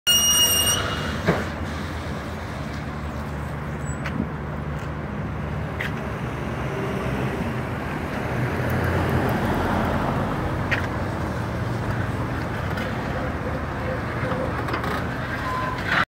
Grúa de tránsito arrastrando auto con freno de mano
Grúa de tránsito intentando acarrear vehículo con el freno de mano puesto.
Al tener el freno de mano puesto, el vehículo es arrastrado, se suelta y es golpeado. Así tratan a nuestro vehícuos...